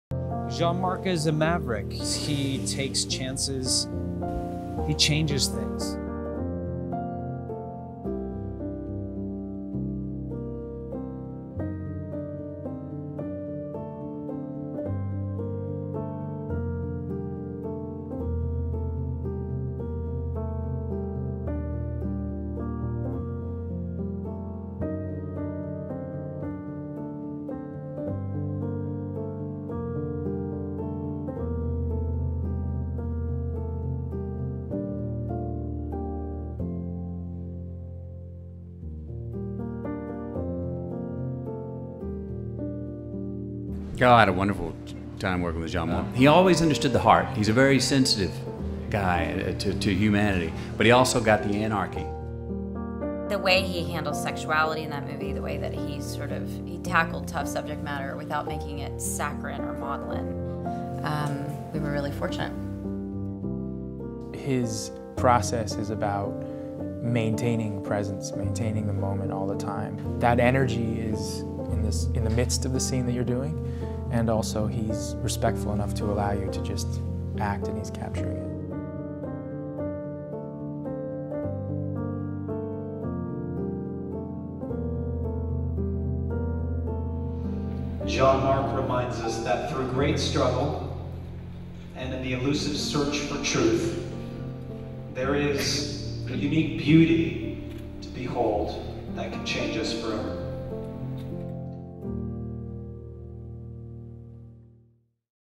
Jean-Marc Vallée, Director of 'Big Little Lies' and 'Dallas Buyers Club', Dead at 58 | PEOPLE
Jean-Marc Vallée, the award-winning Canadian director of acclaimed projects such as 'Dallas Buyers Club', 'Big Little Lies' and 'Sharp Objects', has died. He was 58.

The Oscar-nominated filmmaker passed away suddenly and unexpectedly on Sunday at his cabin outside Quebec City, PEOPLE confirms. A cause of death was not immediately known.